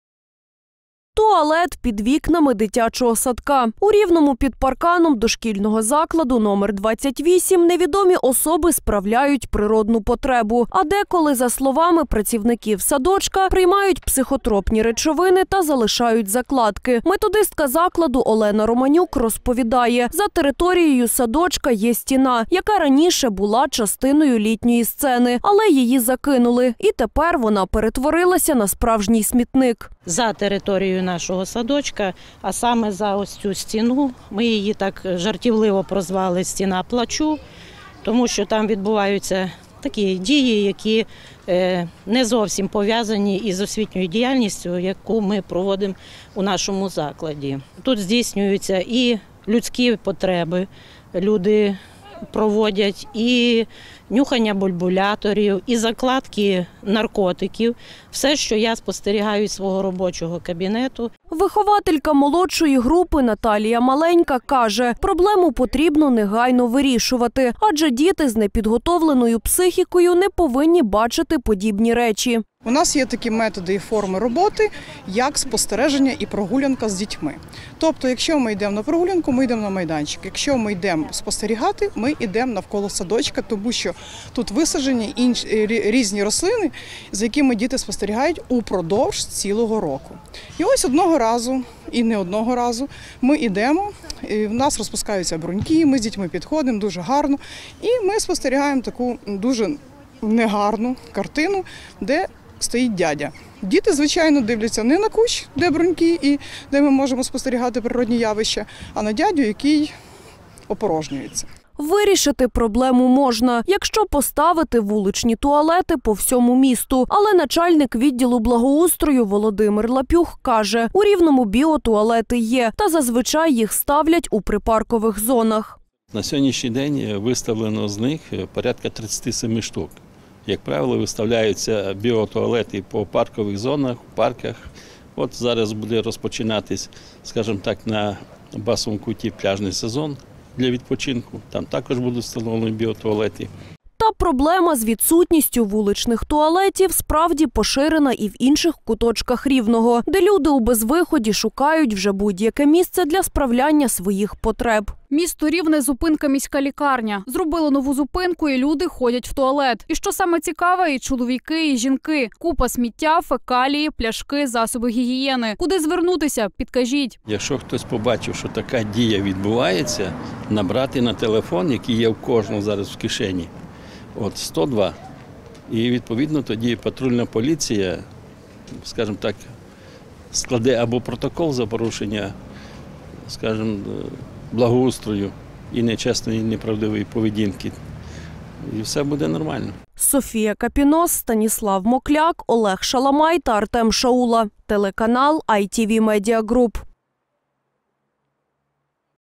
У Рівному невідомі облаштували громадську вбиральню під вікнами садочка
У Рівному вихователі садочка № 28 б'ють на сполох. Під вікнами дошкільного навчального закладу невідомі особи справляють "природну потребу", а деколи й вживають наркотичні речовини. Така аморальна поведінка неабияк лякає працівників садочка, адже це відбувається на очах дітей, які лише починають пізнавати світ. Про деталі цього інцидента та варіанти вирішення проблеми - дивіться у сюжеті далі.